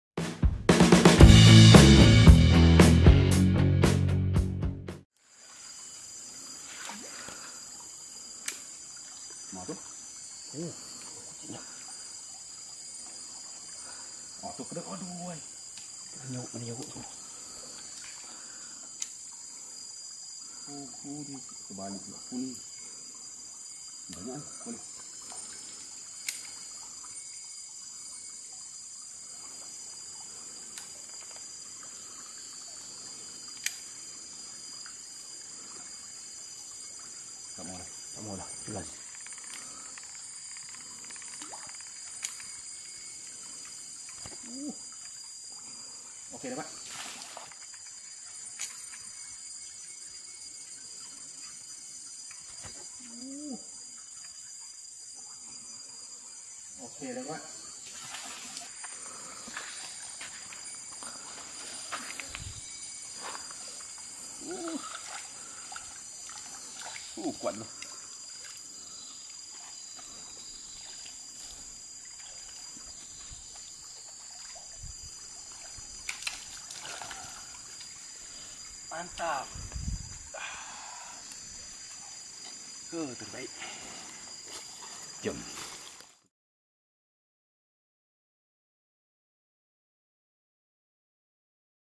SERANGAN PADU SEBARAU MENGENDAP DIBAWAH REBA
Serangan padu ikan SEBARAU MENGENDAP dibawah reba. Umpan yang digunakan silau spoon.

Aggressive attack by hampalabarb on ultralight using Silau

More info on our products at / Info Lanjut Berkenaan Produk Kami di